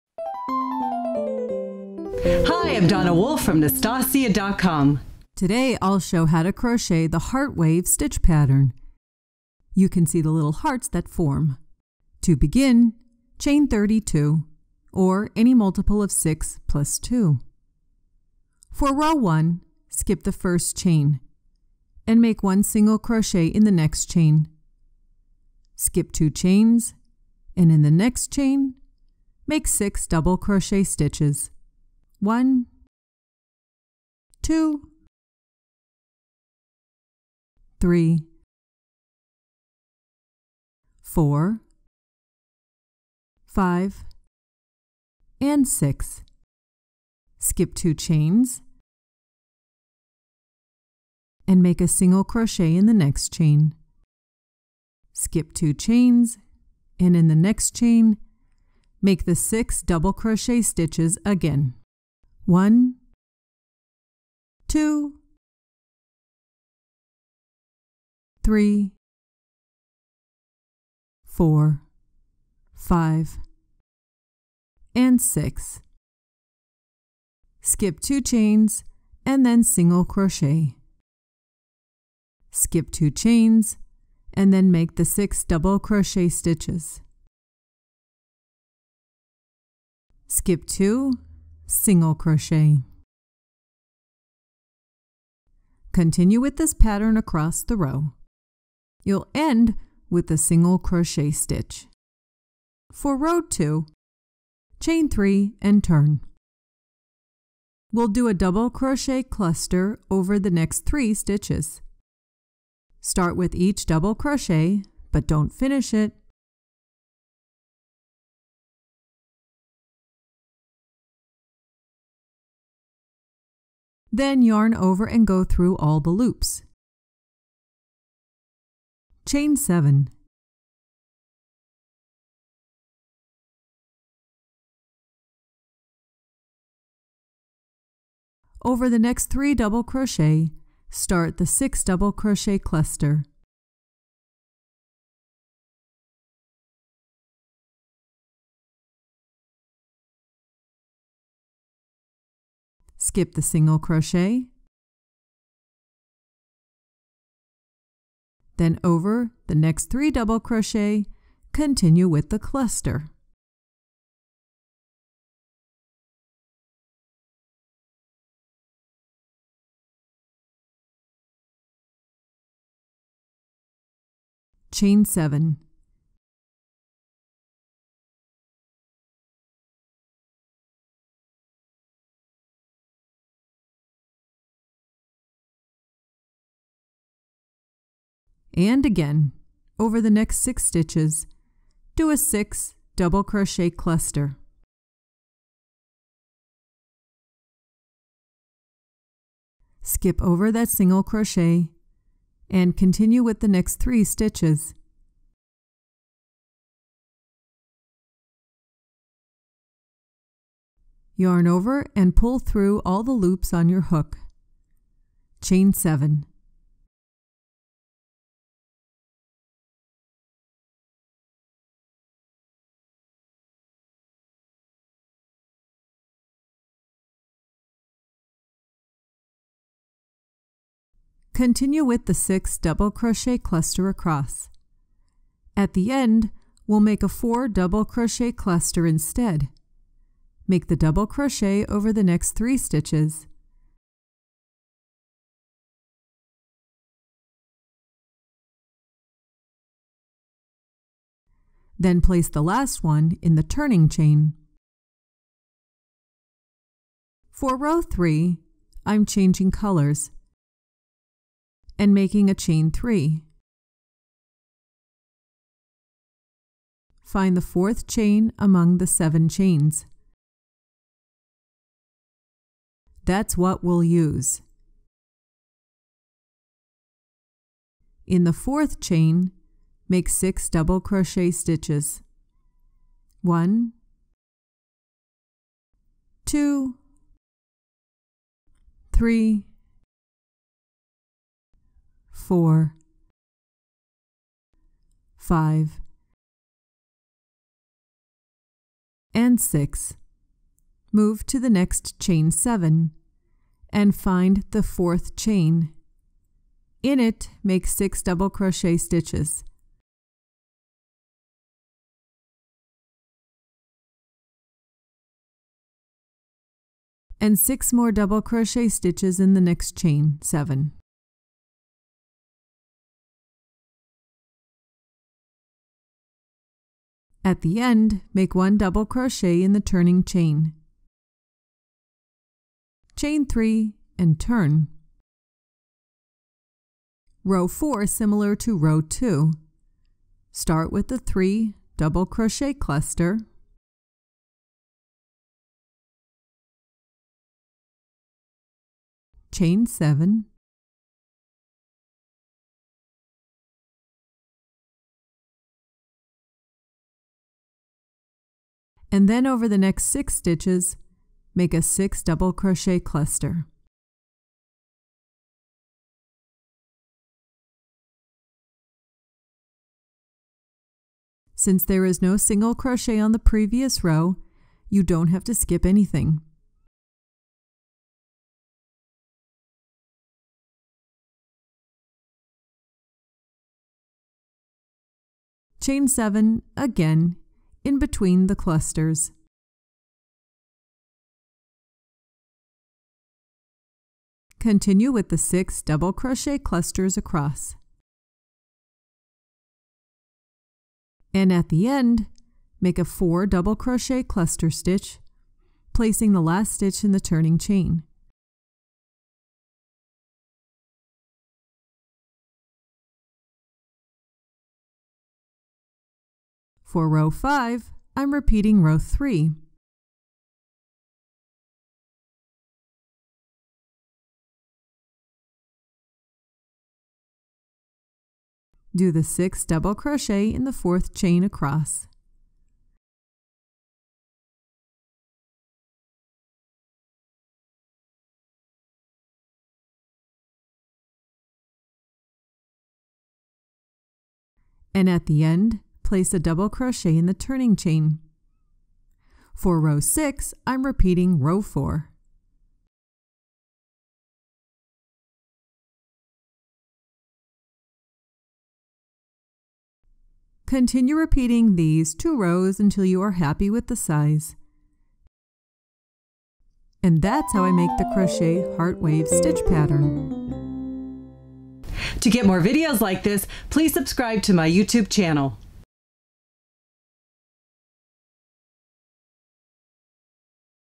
HOW to CROCHET the HEART WAVE Stitch Pattern Tutorial - Little Hearts and Waves
You can learn how to Crochet the Heart Wave Stitch pattern in this tutorial video. Hi I'm Donna Wolfe from Naztazia. This crochet stitch pattern forms little hearts and waves and is lovely for making any crochet items. The pattern is easy to remember and consists of cluster stitches combined with double crochet stitches.